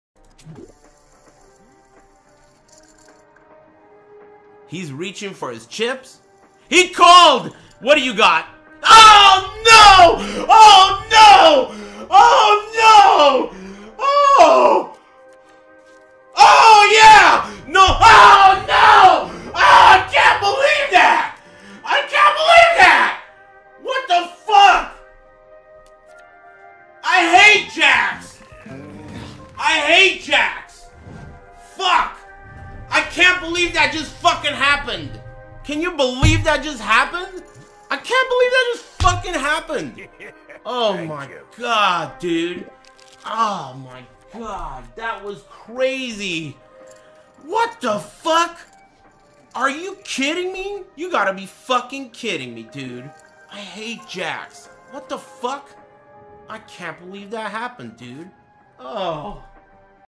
Aris With A Roller Coaster Of Emotions During A Poker Hand
Whats Revival? Haven't i seen you before?

I lost access to my CLIP CHIMPS Account, so this was made. Apologies if you see some of the same clips just reuploading everything then will carry on as usual.